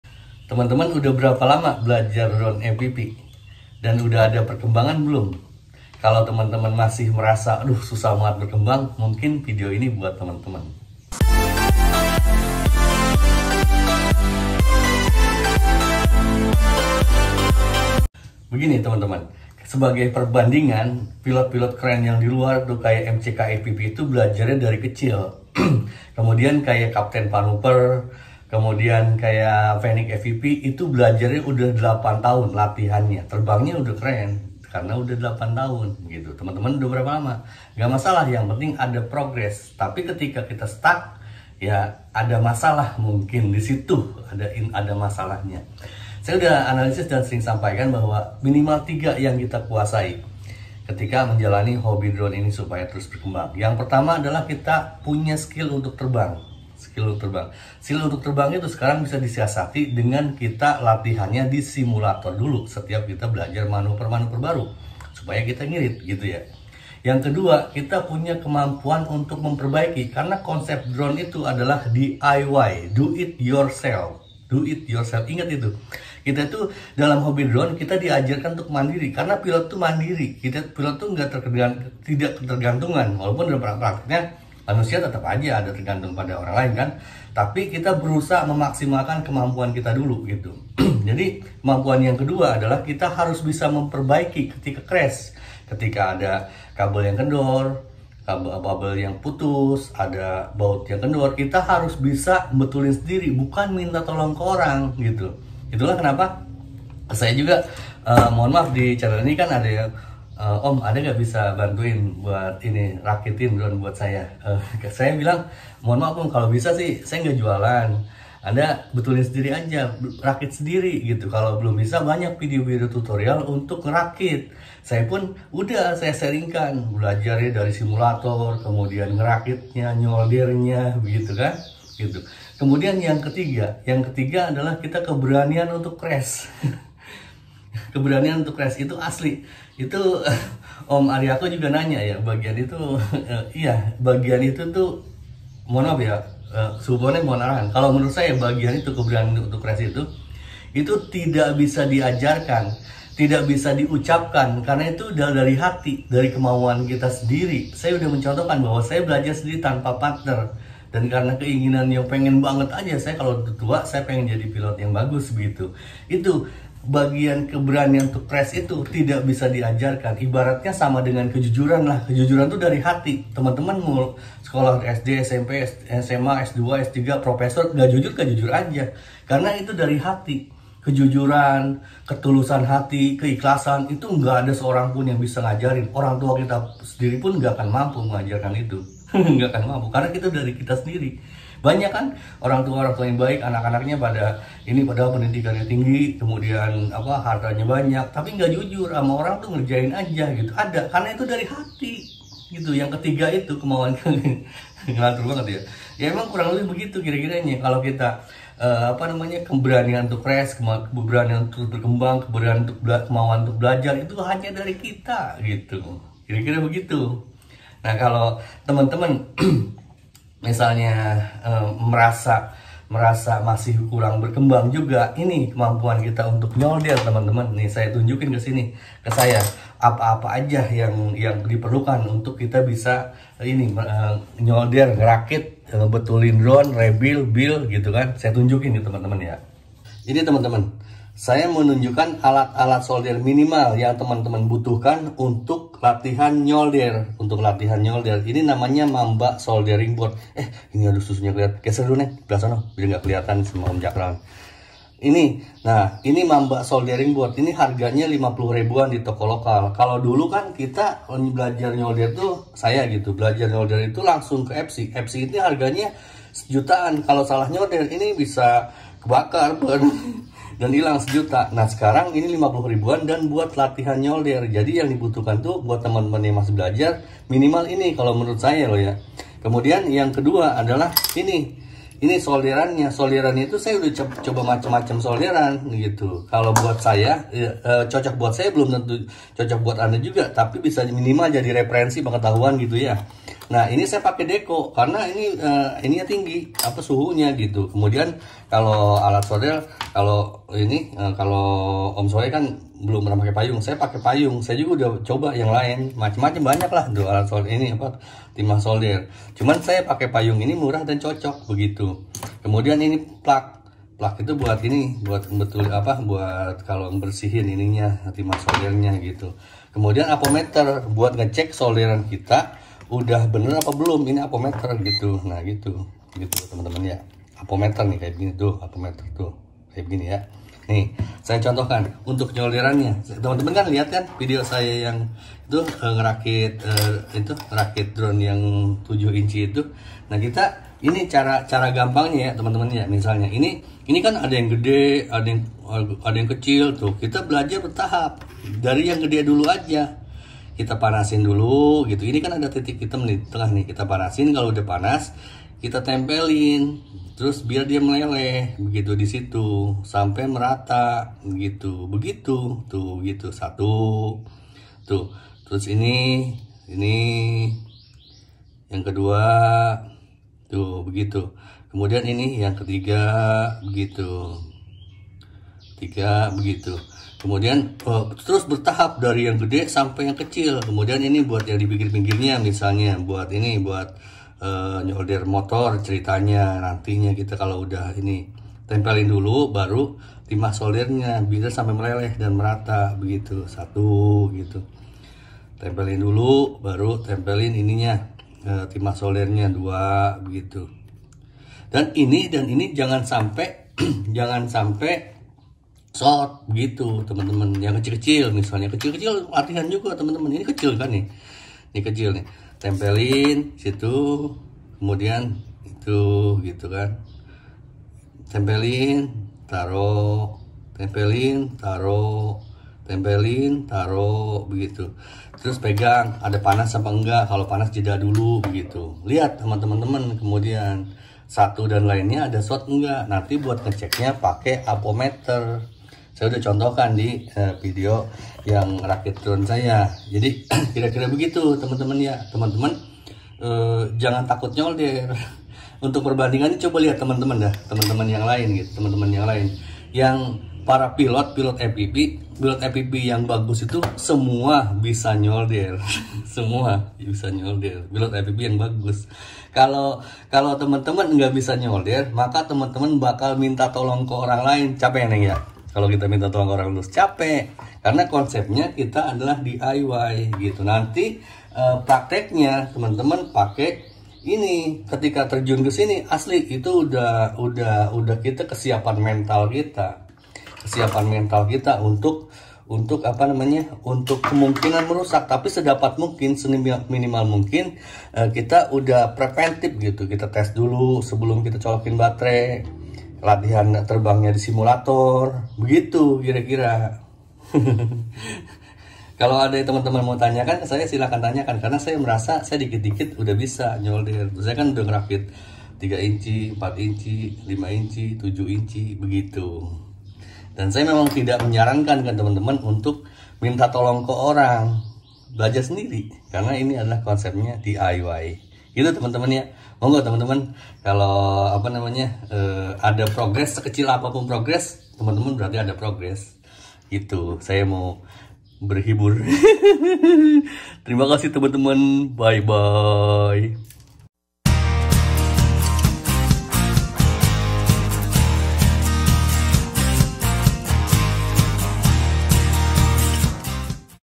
Lama Belajar Drone FPV dan MENTOK? Video ini untuk Anda! (Plus Latihan Solder)😍🤘✈️
Banyak Teman2 yang udah lama belajar Drone FPV tapi merasa MENTOK (kurang peningkatan), pada video ini saya uraikan skill/ kemampuan DASAR yang dibutuhkan agar kita bisa terus berkembang. Saya jelaskan juga prinsip DASAR “pembelajaran” hobby FPV. Saya fikir pemikiran DASAR ini berlaku untuk kita di bidang lain yang kita geluti. Semoga bisa jadi tambahan Pengetahuan untuk Anda, monggo beri komentar. Terima kasih 🙏🇲🇨